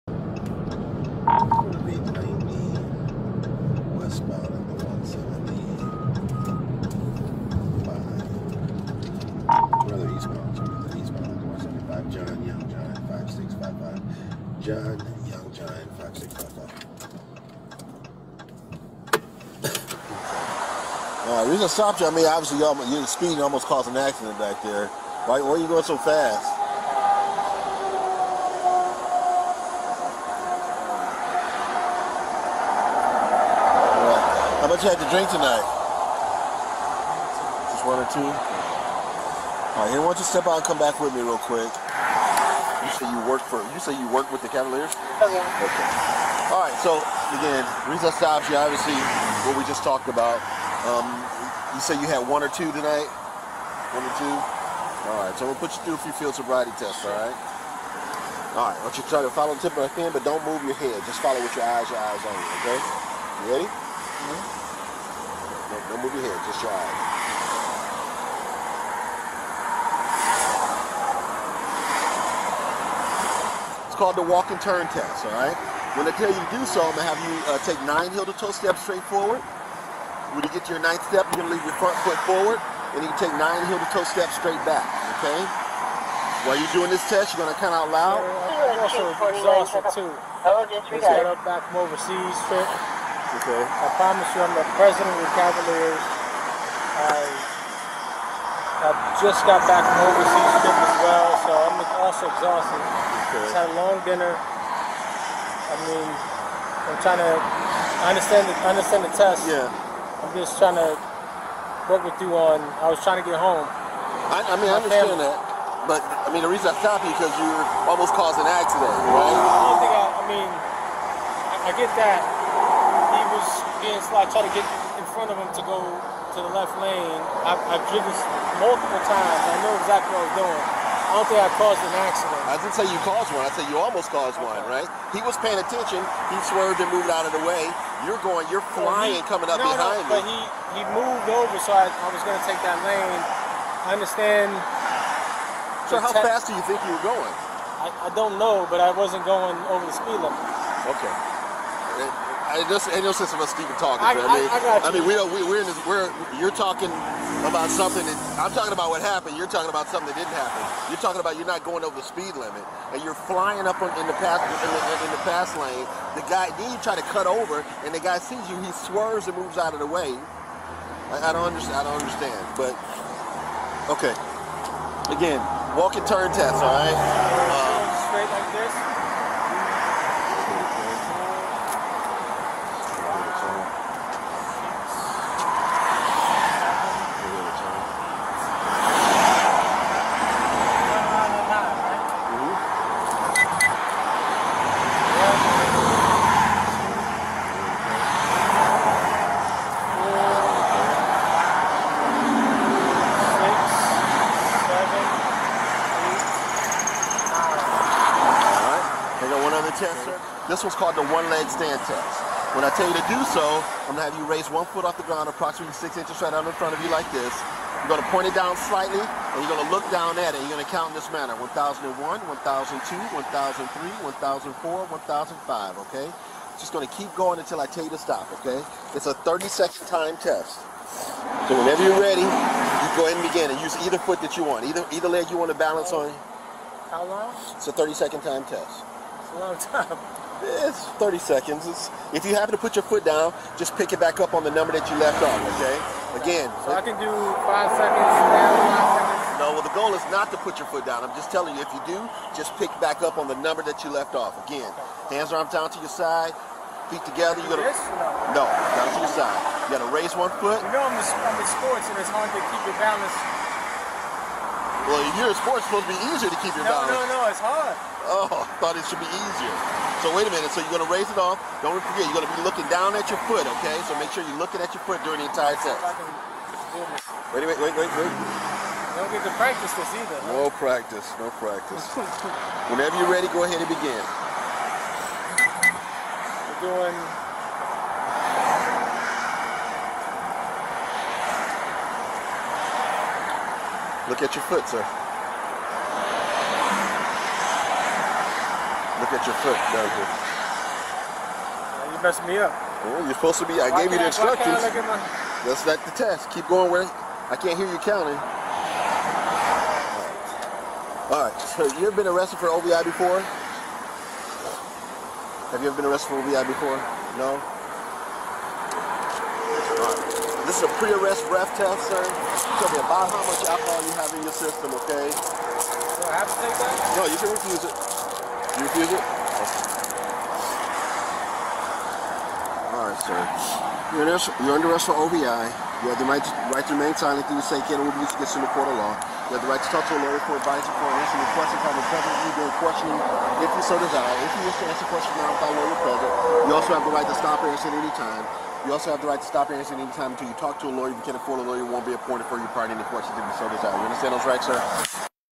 New video reveals details on Cavaliers executive Koby Altman’s OVI arrest
This dash camera and body-worn camera released by the Ohio State Highway Patrol shows what happened leading up to the OVI arrest of Koby Altman, the president of basketball operations for the Cleveland Cavaliers. Video has been edited by News 5.